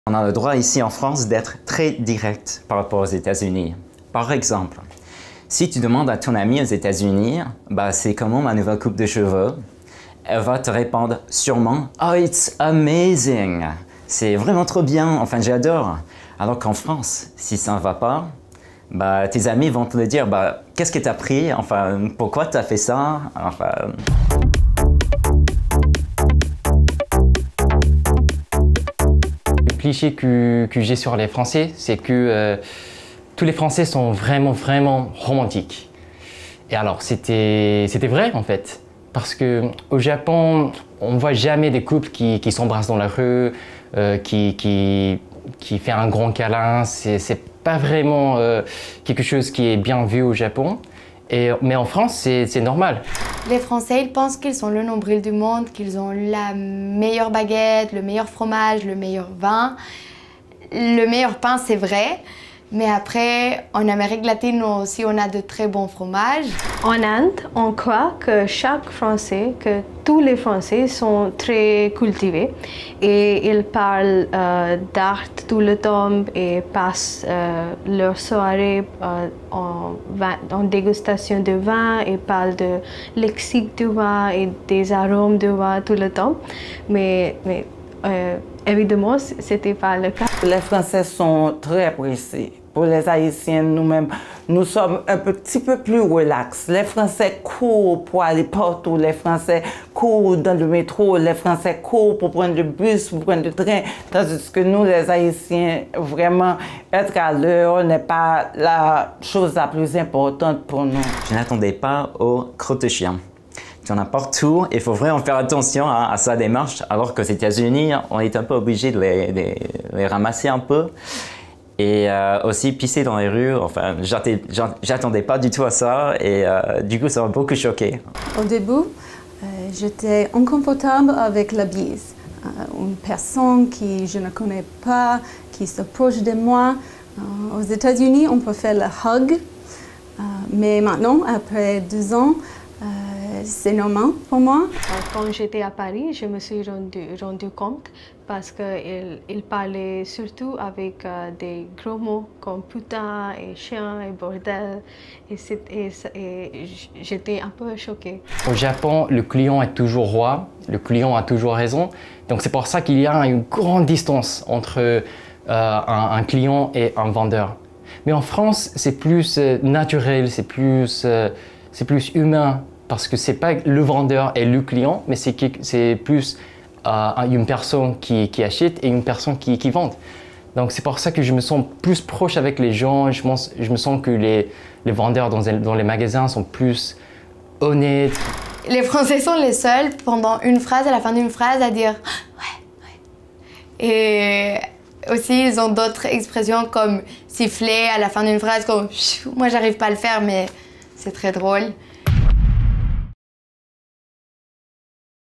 Comment les Français sont vus hors d'Europe
Les Français se prennent pour le nombril du monde, sont sales et tout le temps pressés. Et si ce n'était pas un cliché ? Voilà comment les Français sont vus hors d'Europe… 🇫🇷